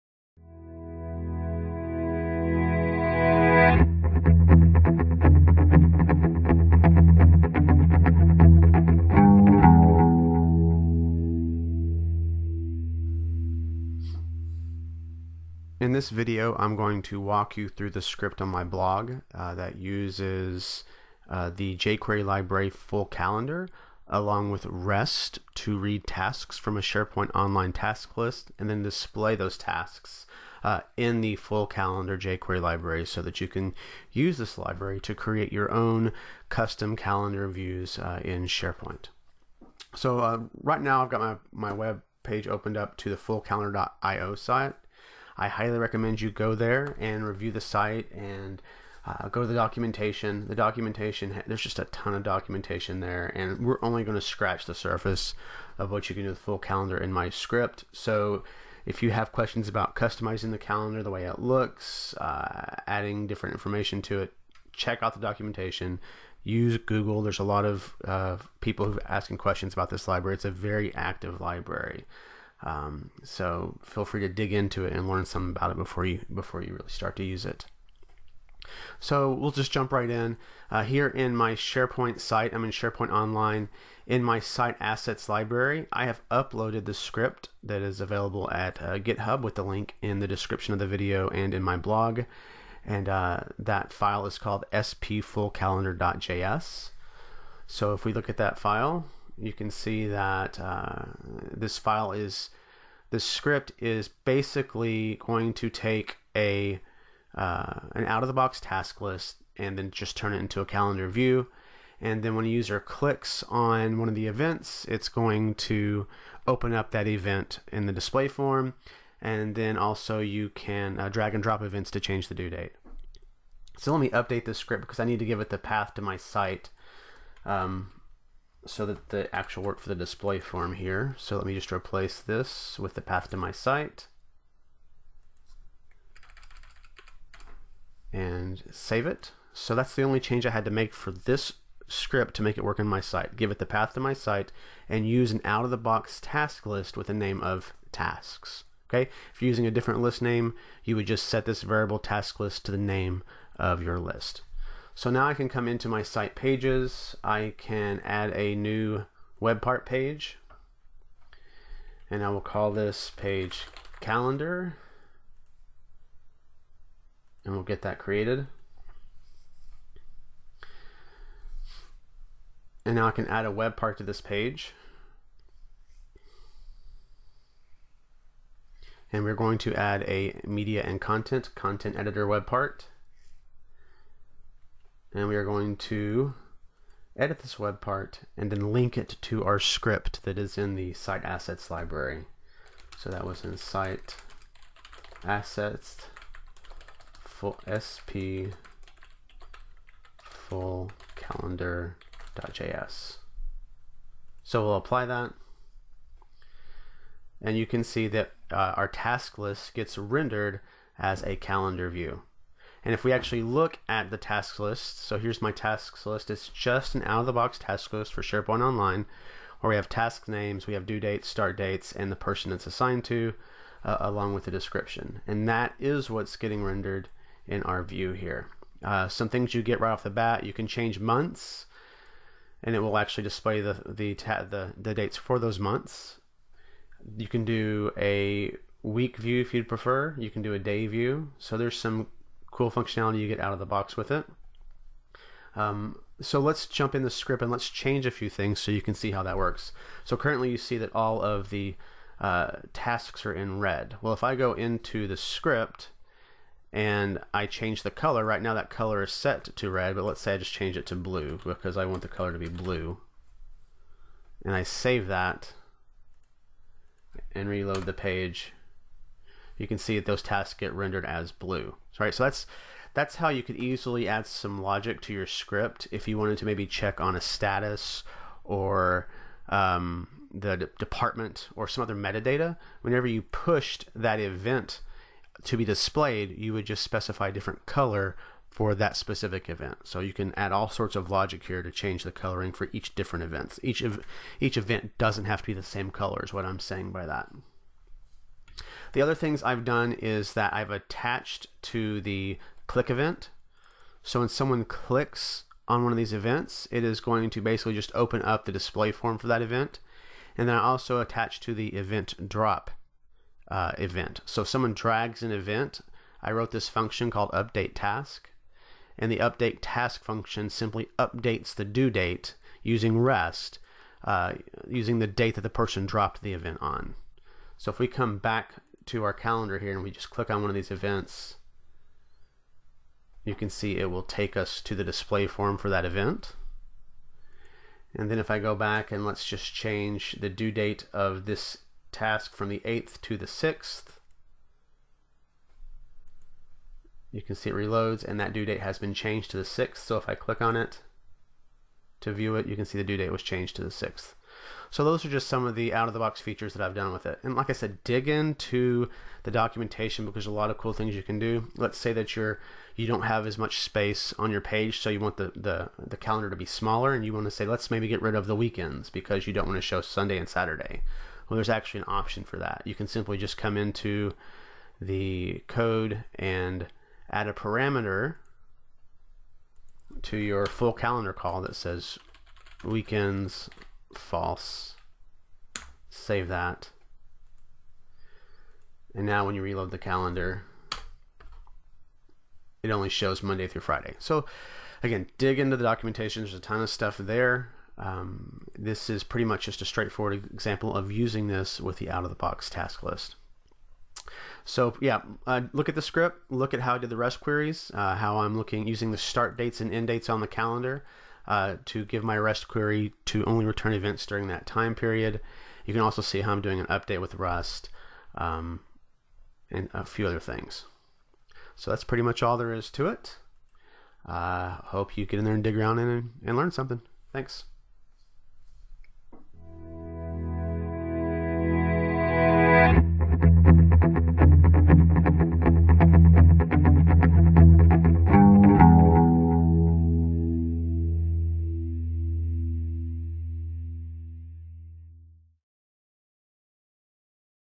Using FullCalendar.io to Create Custom Calendars in SharePoint
Video walking you through the script found at my blog on creating a calendar view using a SharePoint Task List, FullCalendar.io, and REST.  This script also uses a REST update to update the due date for a task when a user drags and drops that task